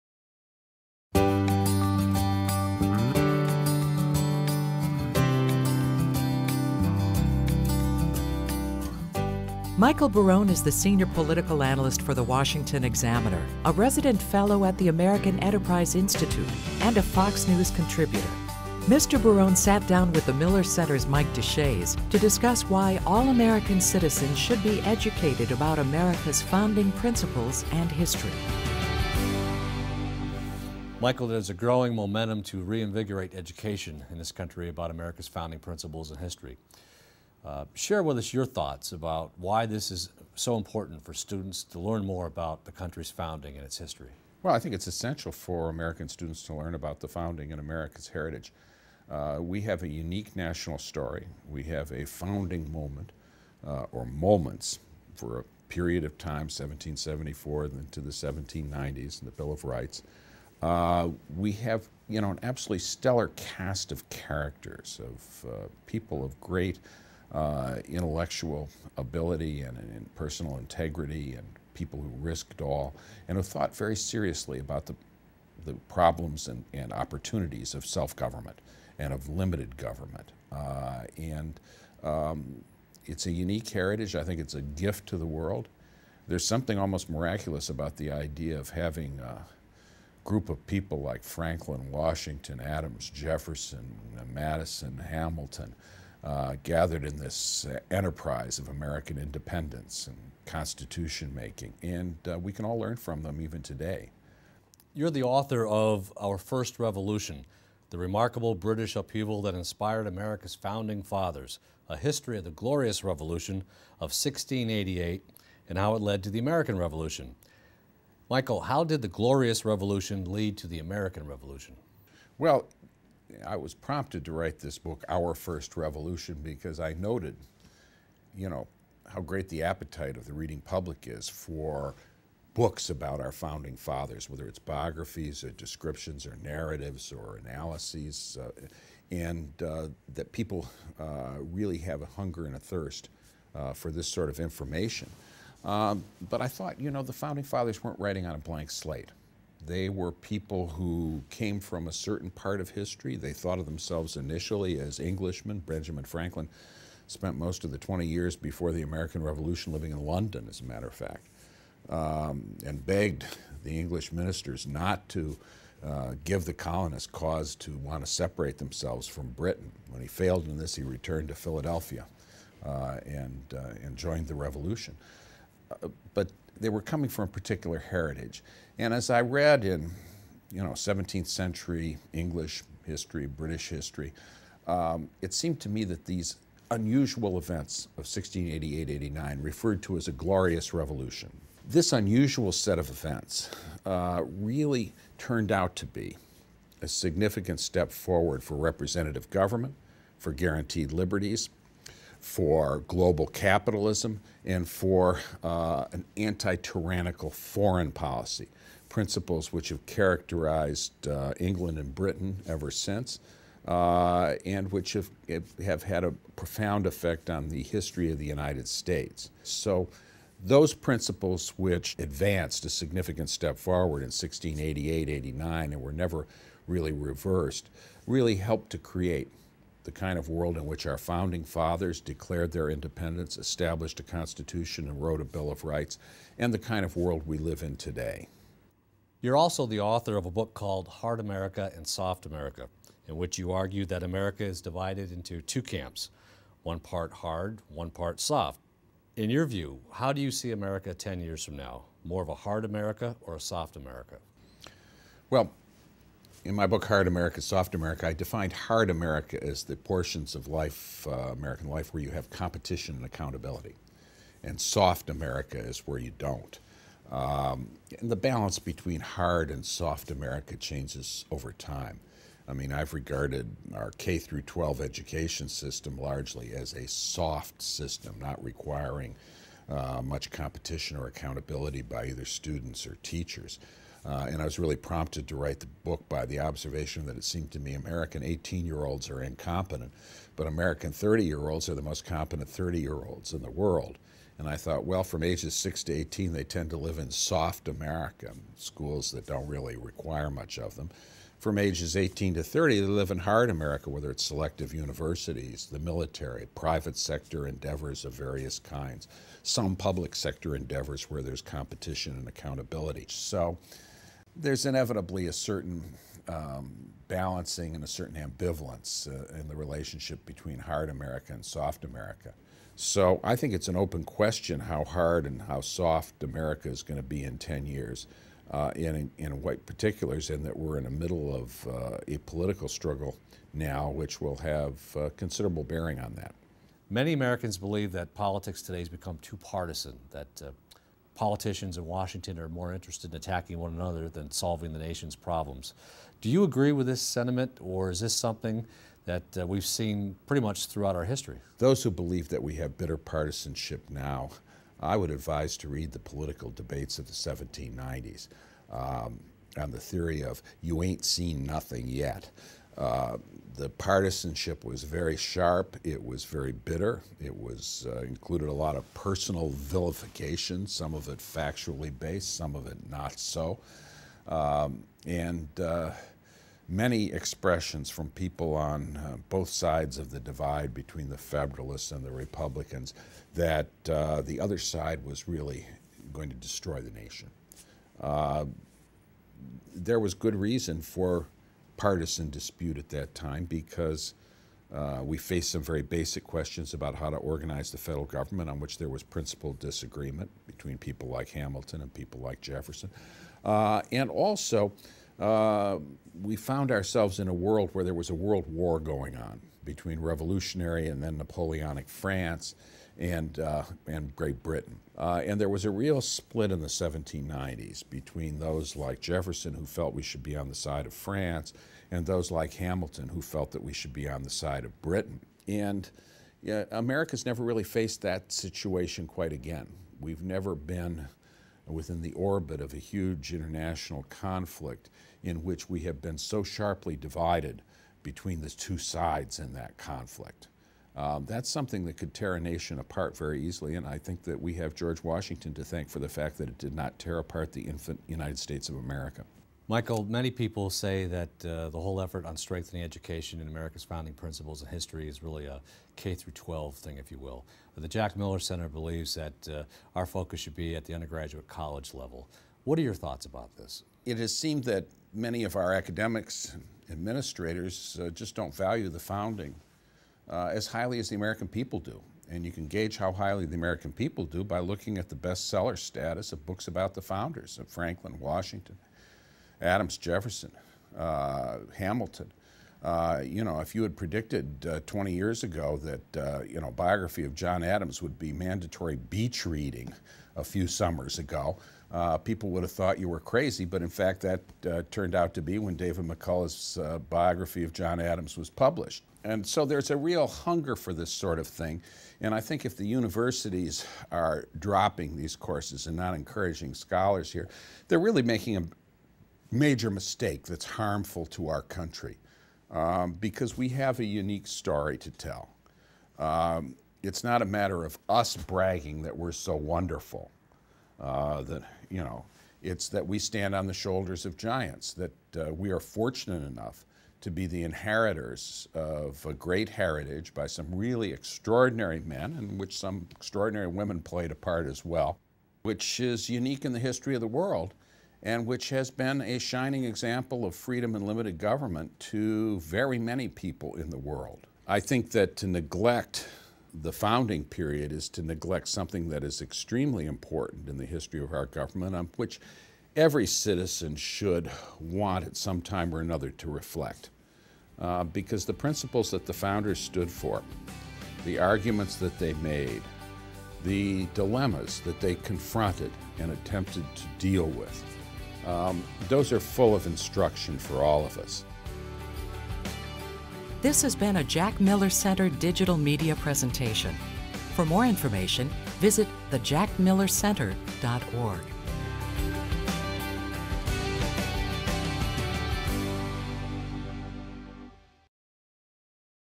JMC Interview Series - Michael Barone
Michael Barone is the senior political analyst for the Washington Examiner, a resident fellow at the American Enterprise Institute and a Fox News contributor.
He sat down with the Miller Center's Mike Deshaies to discuss his books, "Our First Revolution" and "Hard America, Soft America", as well as his view on the importance of teaching America's founding principles and history.